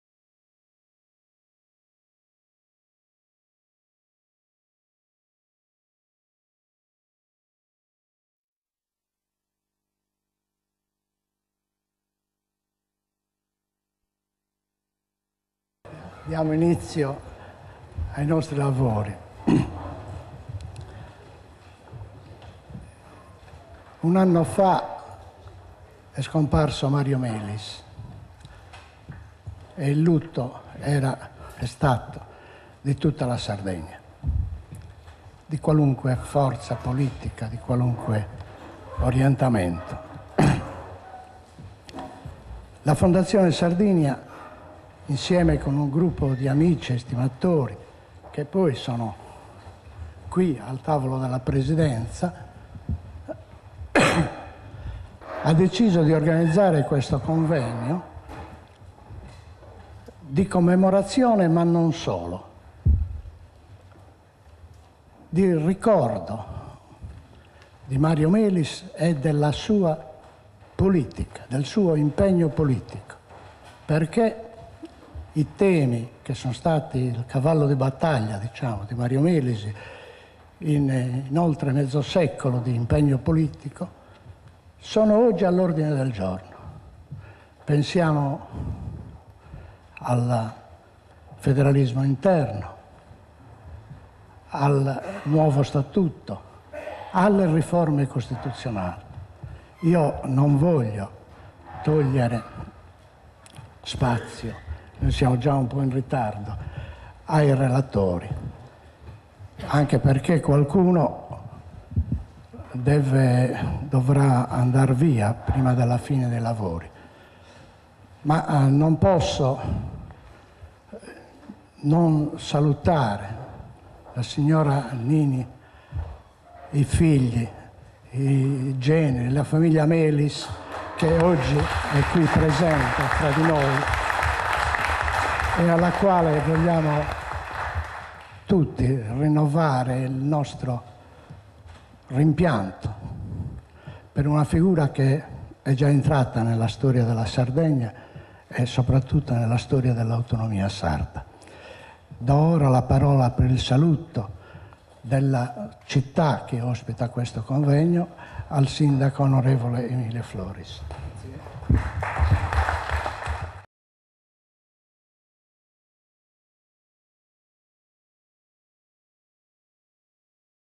04-11-27 video01 AUTONOMIA E FEDERALISMO DI MARIO MELIS introduzione VINDICE RIBICHESU
04-11-27 video01 AUTONOMIA E FEDERALISMO DI MARIO MELIS introduzione VINDICE RIBICHESU
AUTONOMIA E FEDERALISMO
NELL'IMPEGNO POLITICO DI MARIO MELIS
27 Novembre 2004 - Banca CIS